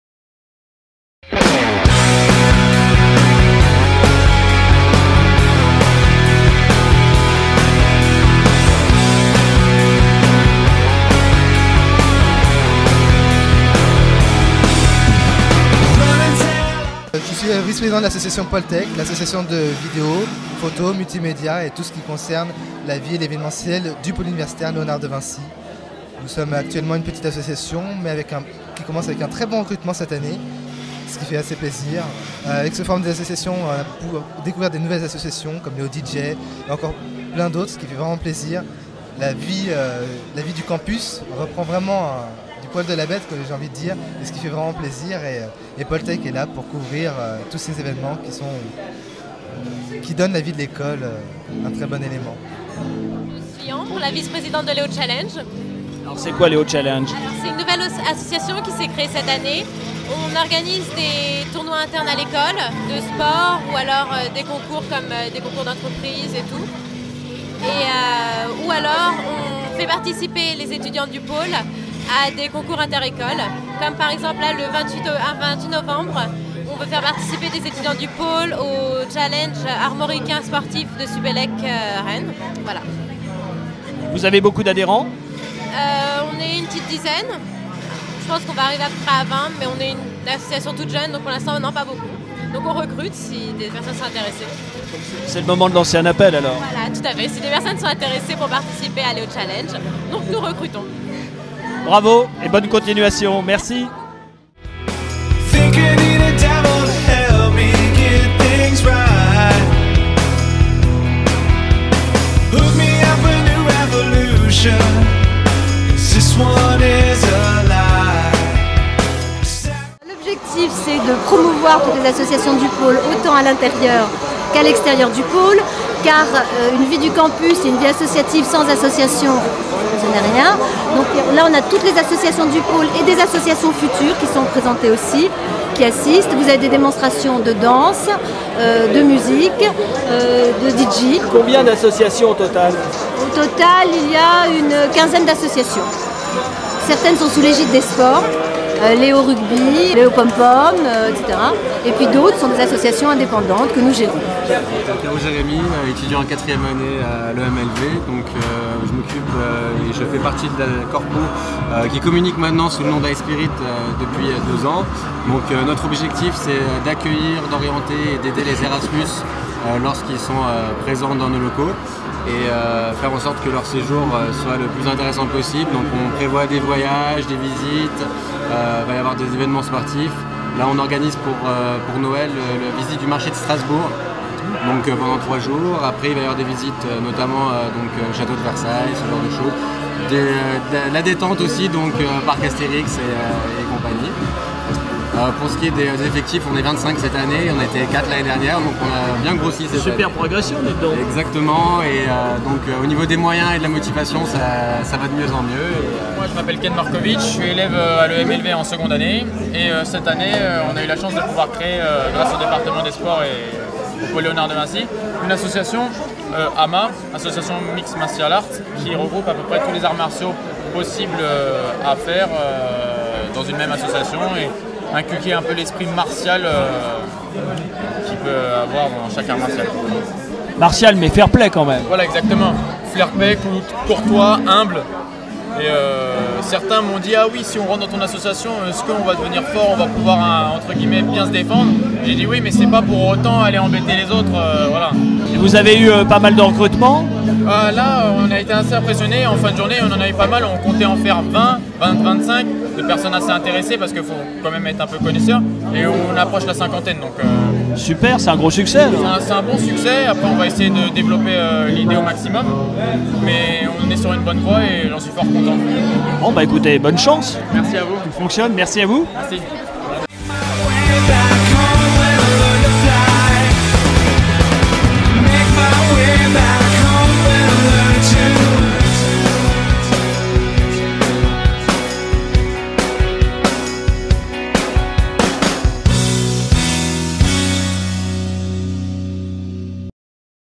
[L'école] Forum des Associations du Pôle Universitaire Léonard de Vinci, campus de l'IIM
Découvrez la vidéo de présentation du forum des associations qui a eu lieu le 10 novembre 2009.23 associations ont animé un stand pour renseigner les étudiants et présenter leurs projets pour l'année.

Associations présentes lors du forum :

- AMMA : Association d'arts martiaux
- De Vinci Finance : Initier les étudiants du Pôle à la finance de marché.
- E-Karting : Association de Karting participant à plusieurs courses chaque année.
- EMLV Consulting : Association de type Junior-Entreprise de l'EMLV spécialisée dans les études marketing et financières.
- La Corpo : Association permettant aux ERASMUS et autres étudiants étrangers de s'intégrer, en répondant à leur questions administratives et personnelles. La Corpo fait aussi des visites organisées.
- Léoaventure : Association de sports extrêmes (Saut en parachute, à l'élastique...).
- Léochallenge : Organisation de rencontres sportives inter-écoles
- Léoconference : Organisation de conférences, débats et joutes oratoires.
- LéoDJ : Association de musique électronique.
- Léogreen : Association sur le thème du développement durable.
- LéoPomPom : Association de PomPom Girls.
- LéoRugby : Association de rugby.
- Léovoile : Association de voile, participant à la Course Croisière de l'EDHEC.
- Le relais des jeunes de Sèvres : Centre à l'écoute des adolescents et jeunes adultes confrontés à des difficultés personnelles, psychologiques, ...
- Les lapins verts : Prévention des addictions
- LKW : Léo King's Walker vous offre la possibilité de faire de la danse.
- MCT : Association pour promouvoir le Master commun Dauphine/EMLV et accompagner les étudiants de cette filière.
- MusiqueMix : Association de musique qui propose des cours et participe à des concerts.
- Poletech : Association de multimédia (Photo, Vidéo, Courts métrages, animation...) se rendant aux évènements relatifs au Pôle.
- Slide Session : Association autour des sports de glisse.
- United Schools of Leonard : Association pour créer des liens entre les différentes écoles présentes sur le Pôle.
- Vinci Comédie : Association de théâtre.
- Zup de Co : Aide aux collégiens de ZEP en difficulté scolaire.